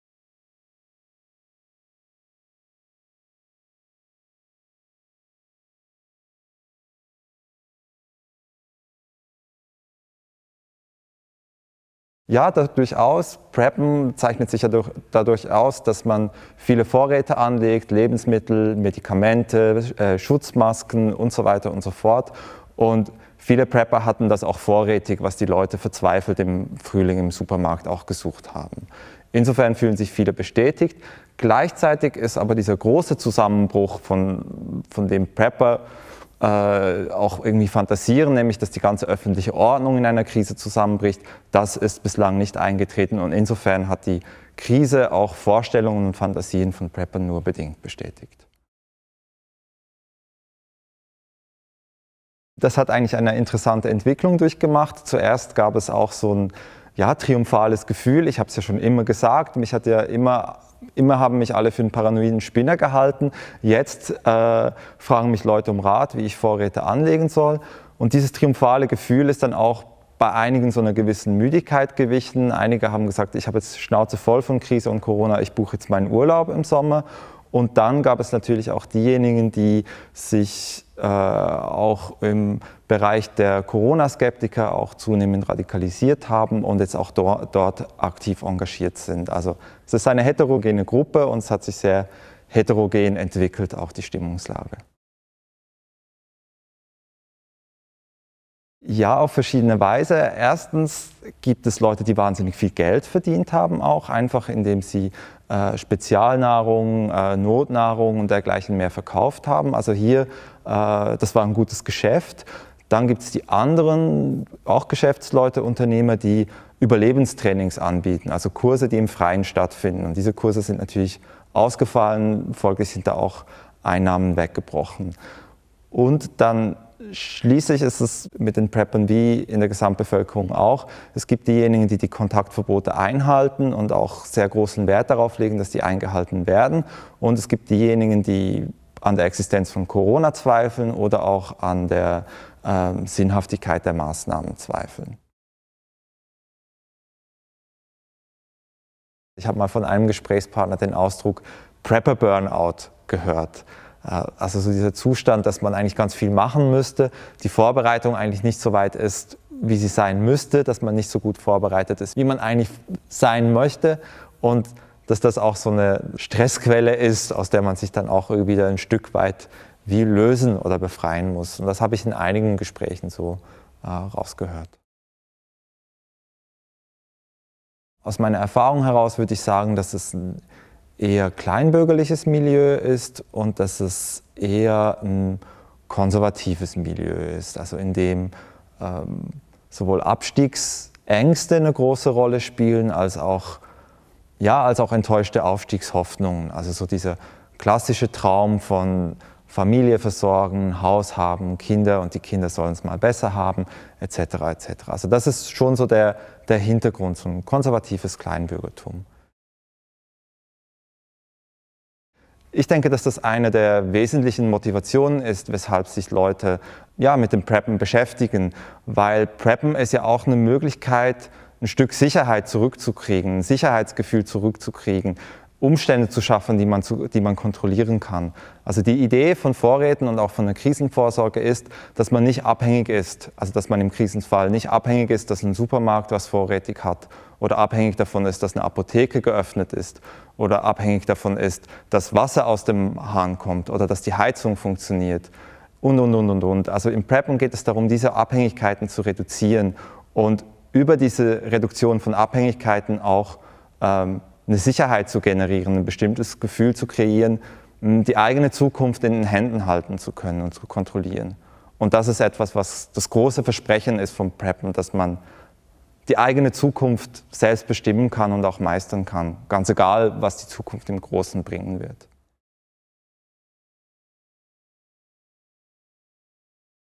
Die Prepperszene in der Pandemie - Julian Genner - Corona im Kaleidoskop
Podcast zur Coronakrise - Uni Freiburg